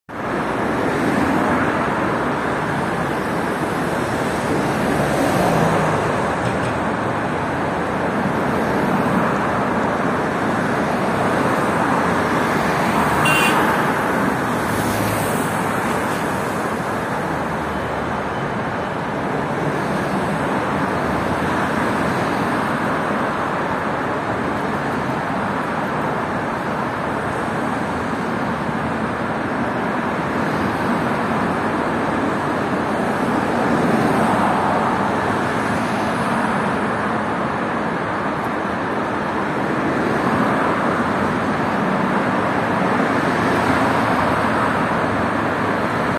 City walks. Moscow, Arbat district. Look what an interesting, beautiful view
City walks. Moscow, Arbat district. Look at what an interesting, beautiful view of the Stalin skyscraper. Cars are reflected in a huge mirror.